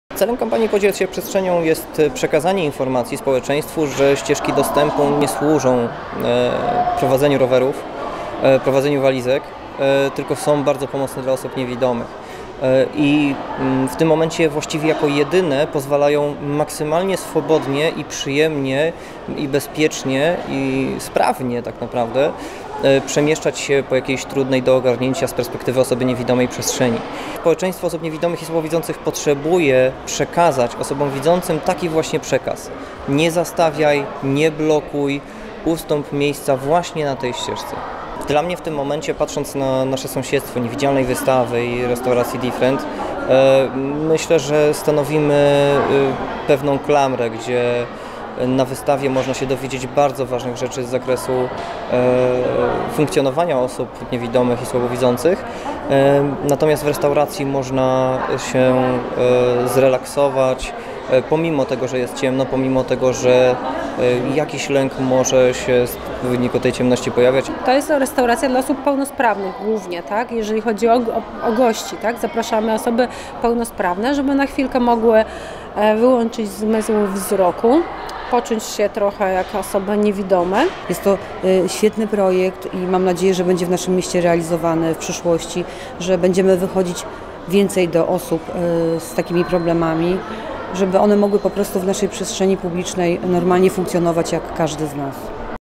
Nie zastawiaj, nie blokuj – podziel się przestrzenią
Wypowiedź: Łukasz Słowik Przewodnik, Niewidzialna Wystawa Warszawa, Anna Bocheńska Prezes Fundacji Pomocy Rodzinie „Człowiek w potrzebie”, Gabriela Szustek Rada miasta st. Warszawy

Z pewnością kojarzymy występujące w metrze, na dworcach, a nawet na ulicach srebrne linie. To przygotowane specjalnie ścieżki doprowadzające. Ich zadaniem jest ułatwić poruszanie się osobom niewidomym. Nie wszyscy o tym wiedzą. Niestety, większość osób widzących nie wie do czego one służą i stawia na nie walizki albo pozwala się tam bawić dzieciom. Można to zmienić – podziel się przestrzenią!

Mamy pewne standardy dostępności budynków użyteczności publicznej dla osób z niepełnosprawnościami i nie tylko. Ktoś może powiedzieć, że to drobnostka o tym jak traktowane są te ścieżki, celem akcji ścieżkadostępu – podziel się przestrzenią! jest uświadomienie, że nie jesteśmy sami a same ścieżki nie są ozdobą i mają swoje przeznaczenie. Niektórym wydaje się, że jest to tor do ciągnięcia walizek, innym, że „miejsce zabaw” dzieci. Tymczasem są to… ścieżki prowadzące ich zastawienie może mieć smutny finał w postaci złamanej laski czy obrażeń ciała. Poszkodowane mogą zostać także bawiące się w tych miejscach dzieci.

Od ponad pół wieku, na świecie obchodzimy święto osób niewidomych i niedowidzących. 15 października to Międzynarodowy Dzień Białej Laski. W Warszawie w połowie października będzie szereg wydarzeń związanych nie tylko z tym dniem, ale z szeroko rozumianą dostępnością.

Jednym z nich było śniadanie, które odbyło się w restauracji Different. Goście jedli w ciemnościach i byli obsługiwani przez niewidomych kelnerów.

Po śniadaniu goście mieli okazję wyruszyć w wyjątkowa interaktywną podróż w niewidzialny świat, podczas której mogli sprawdzić jak radzić sobie w codziennych sytuacjach bez pomocy wzroku – wyłącznie za pomocą zmysłu słuchu, dotyku, węchu… Pomysłodawczynią zarówno „Niewidzialnej wystawy” jak i samej akcji ścieżkadostępu – podziel się przestrzenią! Jest Małgorzata Szumowska, dyrektor stołecznej „Niewidzialnej wystawy”